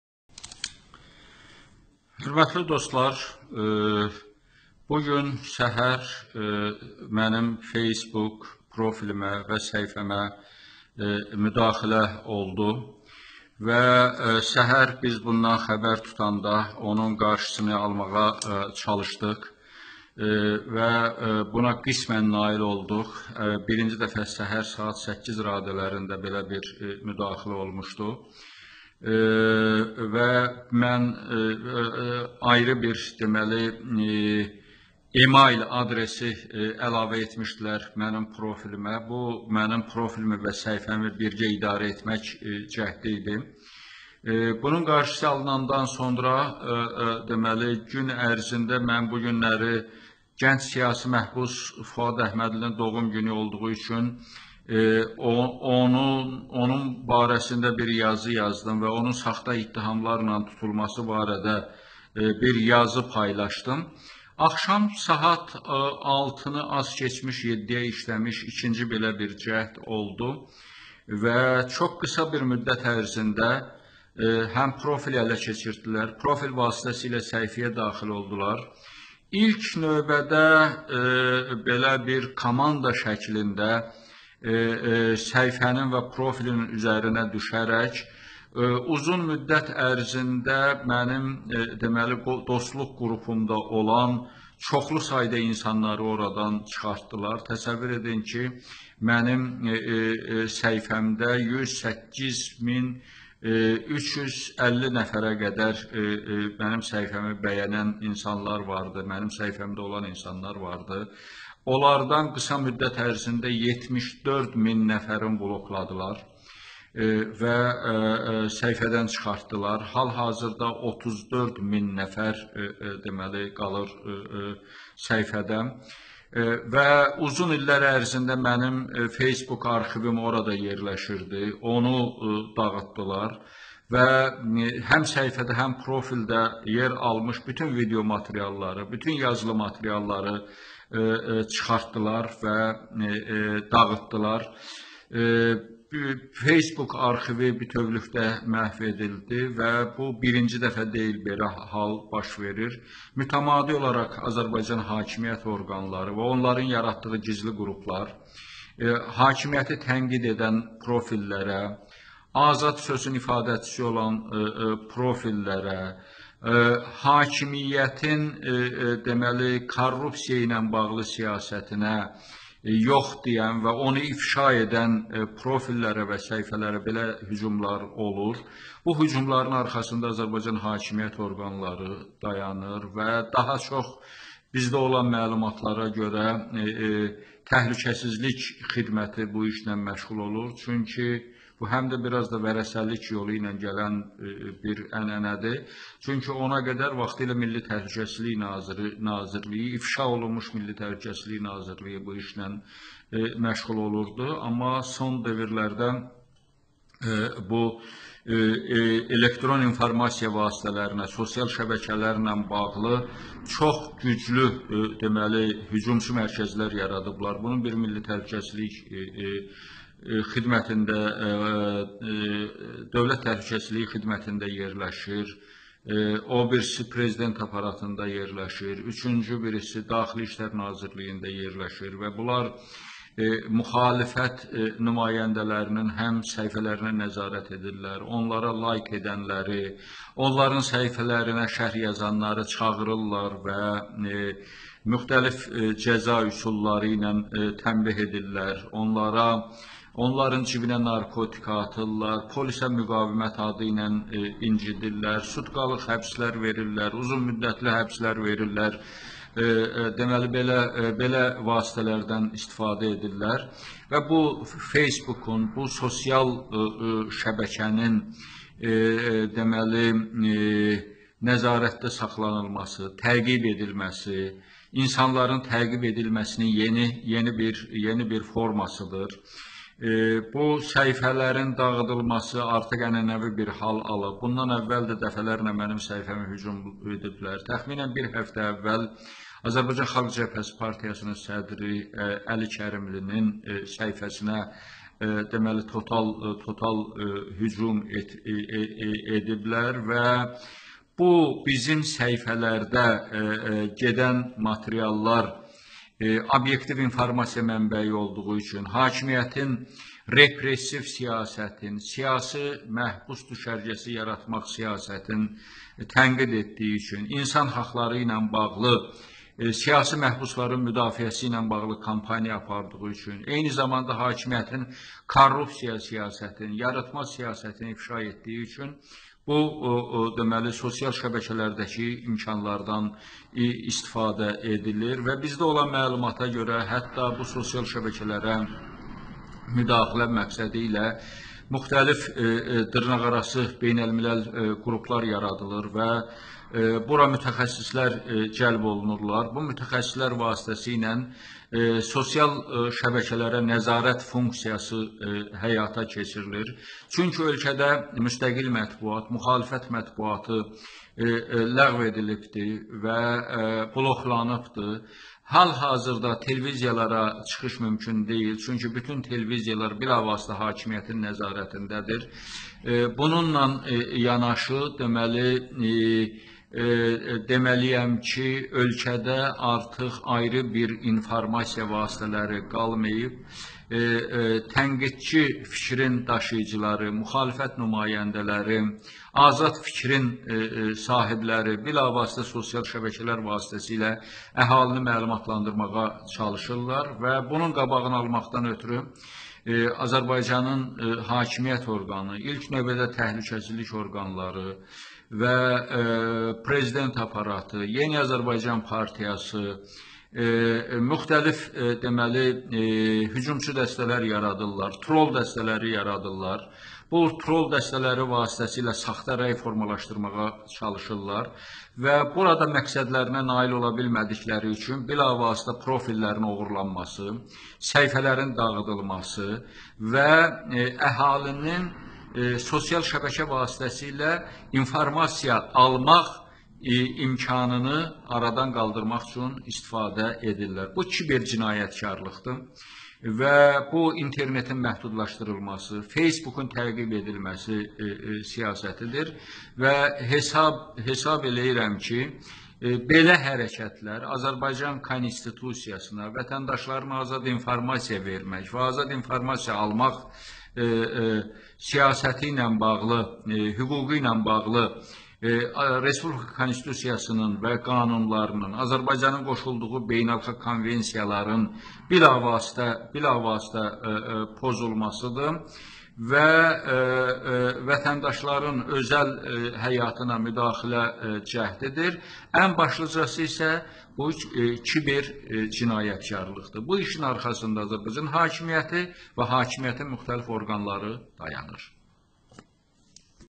Milli Şuranın cədri Cəmil Həsənlinin FACEBOOK səhifəsi yenidən oğurlanib..
Milli Şuranın cədri Cəmil Həsənlinin FACEBOOK səhifəsi yenidən oğurlandı..